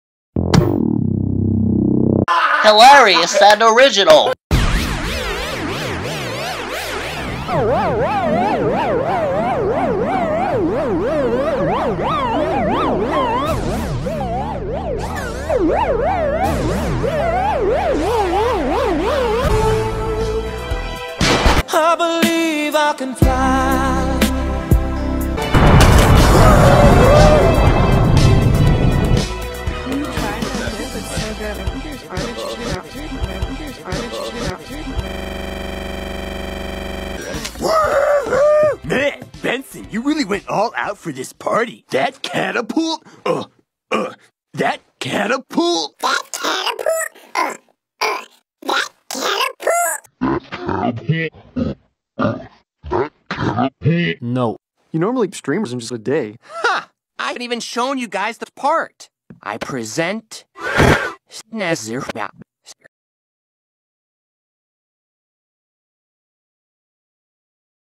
Mini YTP - Bensauce Forgets About Racki the Wishmaker (Collab Entry)
My entry for the Terror Tales of the Park collab by SnoopyTheFudge.